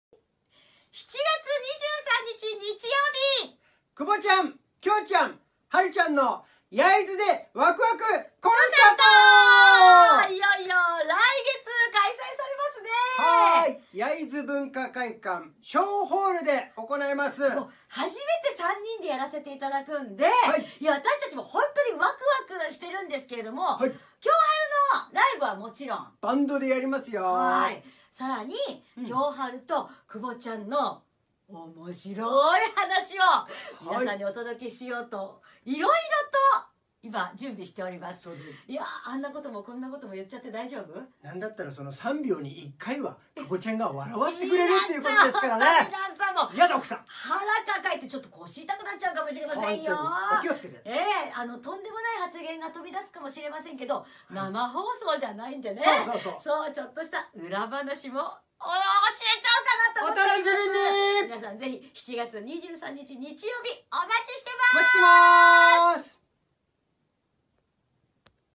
久保ちゃん、京ちゃん、晴ちゃんの、焼津でわくわくコンサート【出演者メッセージ】
京太郎さんと久保ひとみさんからメッセージをいただきました。
当日は京晴によるライブのほか、3人の楽しいトークをはじめ、
食レポコーナーやじゃんけん大会など盛りだくさんの内容でお届けします！

公  演  名◇久保ちゃん、京ちゃん、晴ちゃんの、焼津でわくわくコンサート
公演日時◇2023年7月23日（日）15：00開演
会　　場◇焼津文化会館 小ホール
入  場  料◇全席指定一般2,000円、高校生以下1,000円
　　　　　※3歳以上有料。2歳以下は保護者1名につき1名ひざ上鑑賞可。
　　　　　　ただし、お席が必要なお子様は有料。
出  演  者◇久保ひとみ、京太朗と晴彦

チケットのお申込みは下記URLまたは焼津文化会館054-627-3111まで